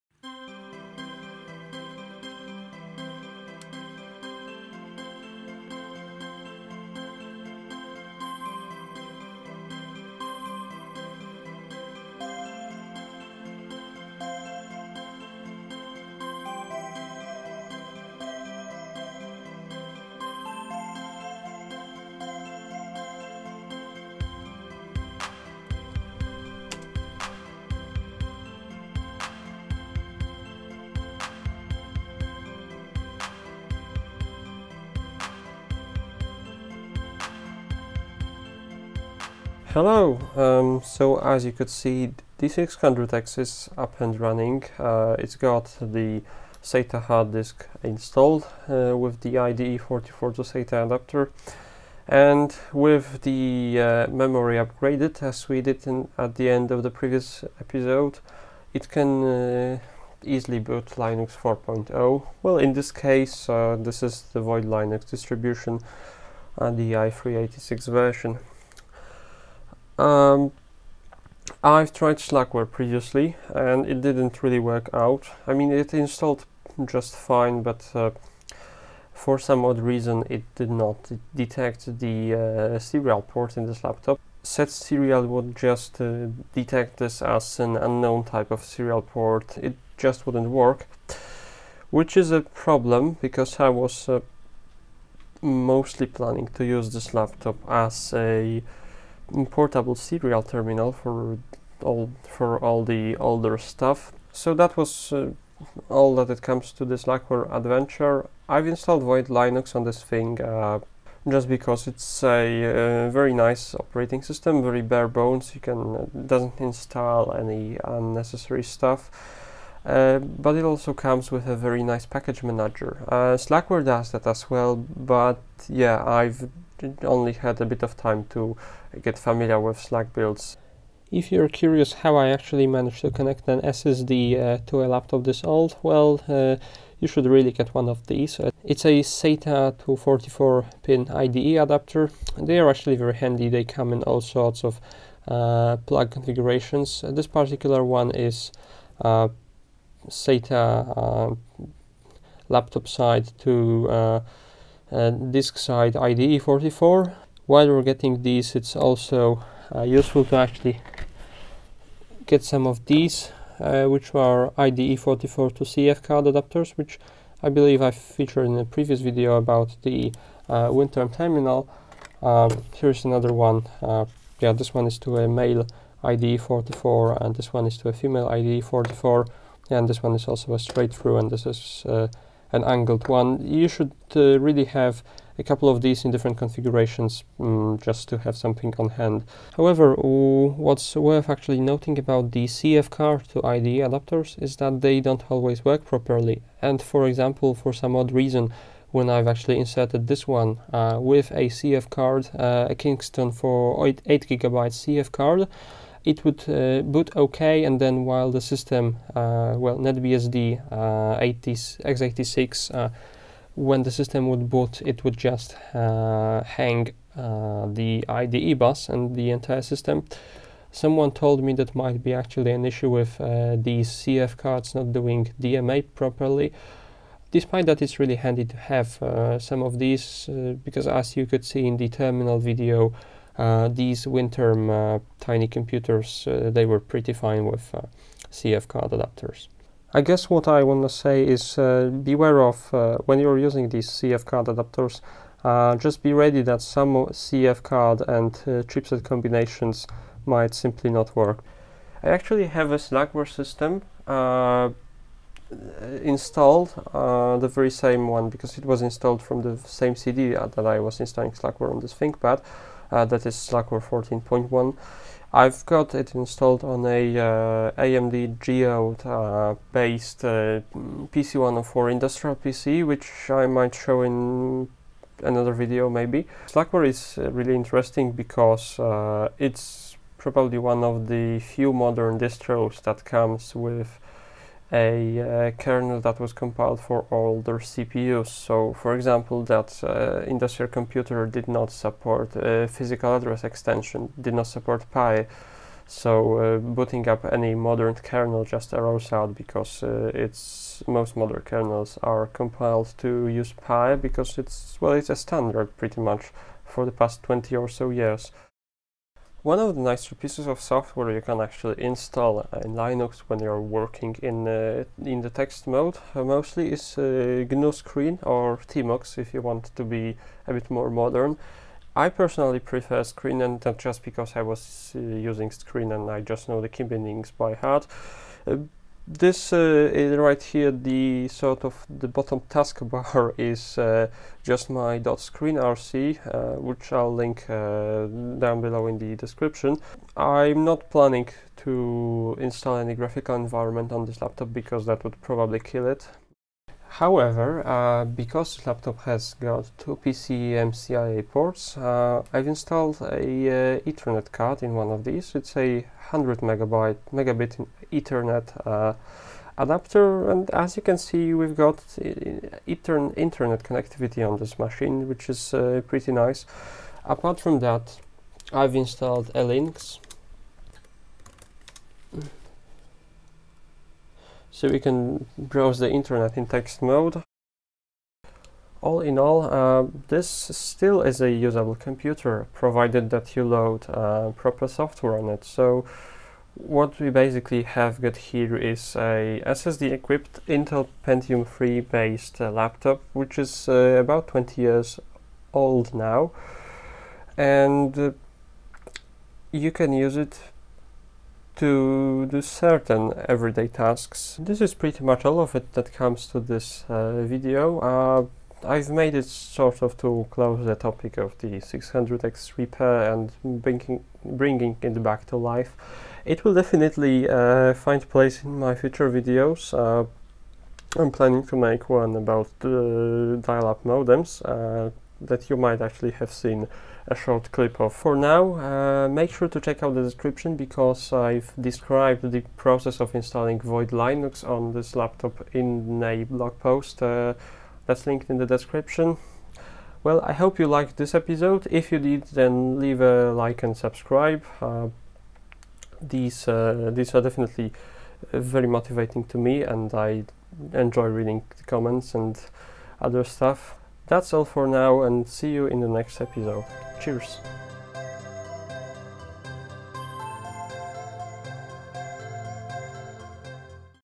PEV 12 - Void Linux, Thinkpad 600x repair pt. 2
The second part of the 600x repair. In this video I show how I run Void Linux off the SSD.